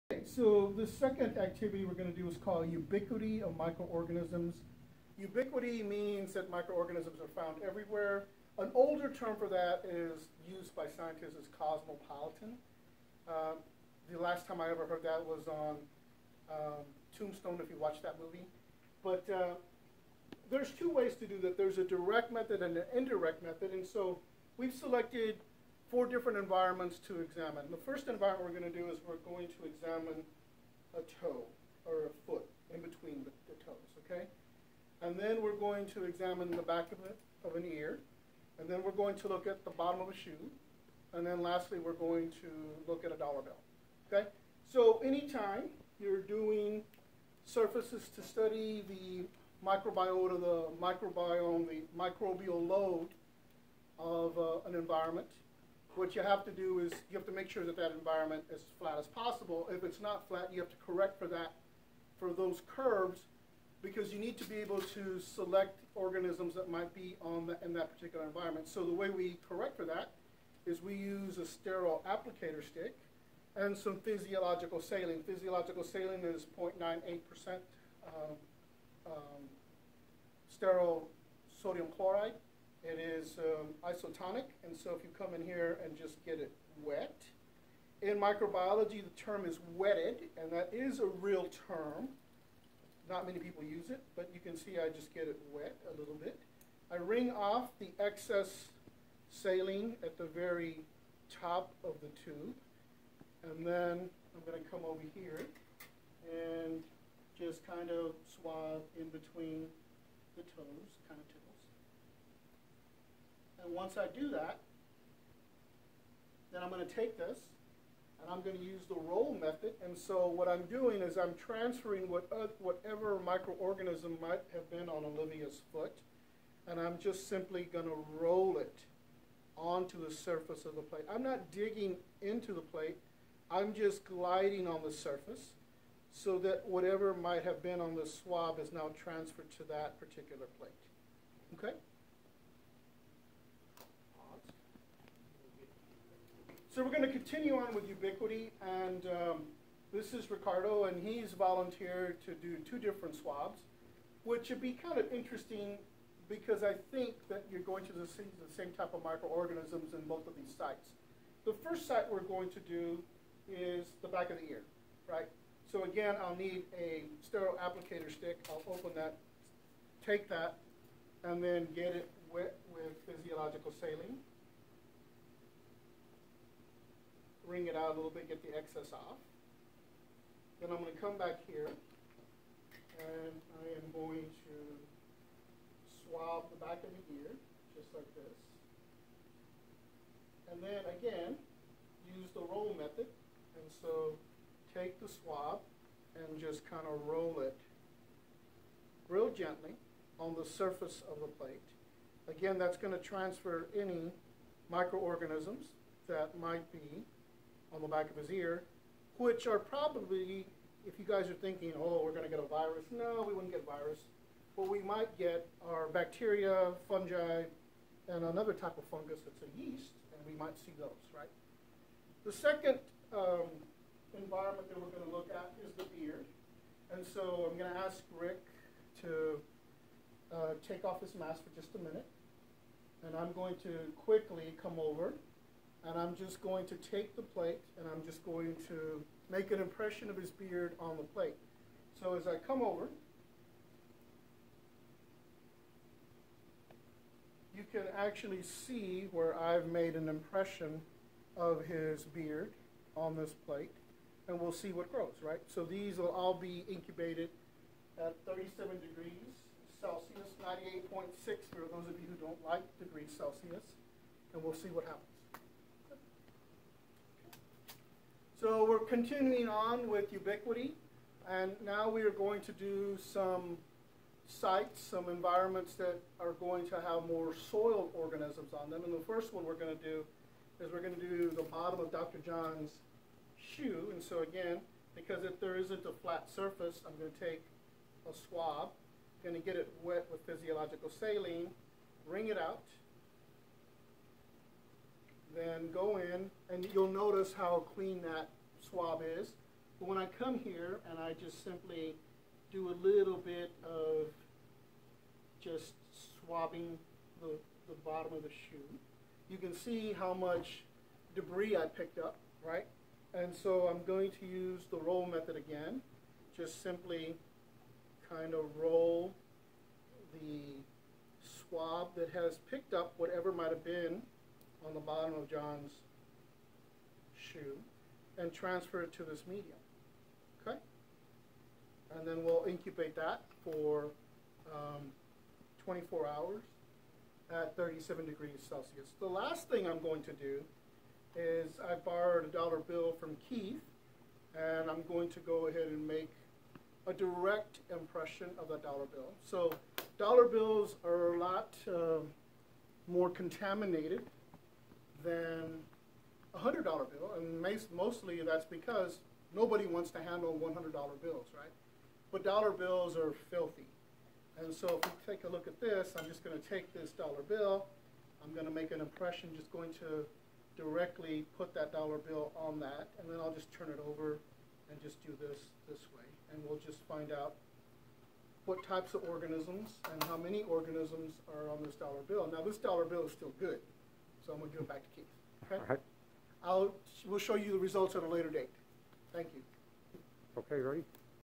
ACC Microbiology Microbial Ubiquity
The video explores the microorganisms found in different environments.